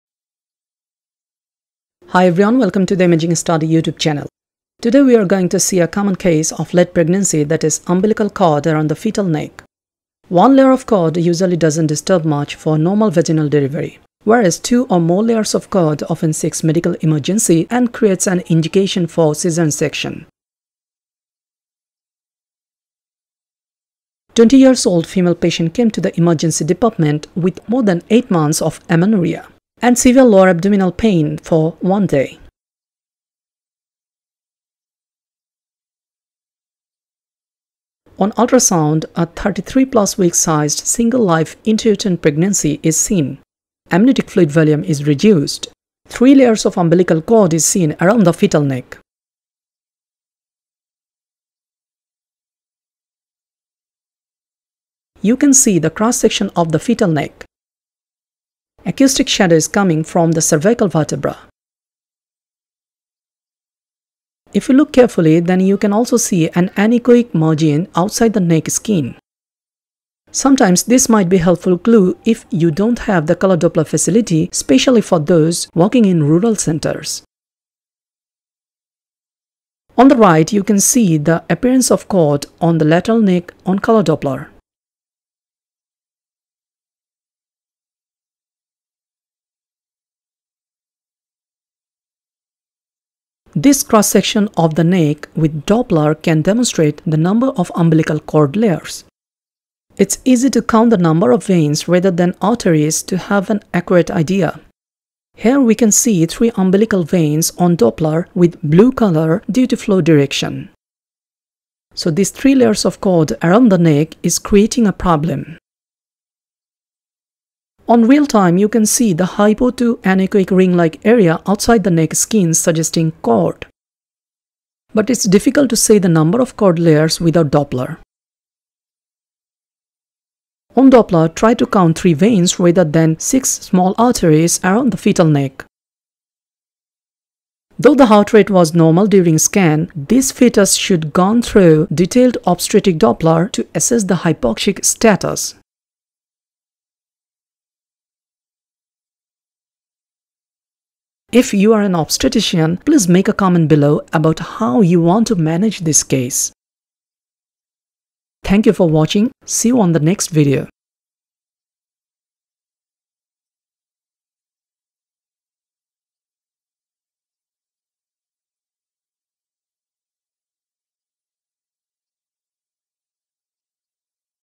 Three Layers of Umbilical Cord around the Fetal Neck || Ultrasound || Doppler || Case 36
Clinical History:
20 years old female patient came with
- Amenorrhoea for 8+ months
- Severe lower abdominal pain for 1 day

Ultrasound Findings:
- 33+ weeks sized single live fetus
- 3 layers of cord around the neck
- Oligohydramnios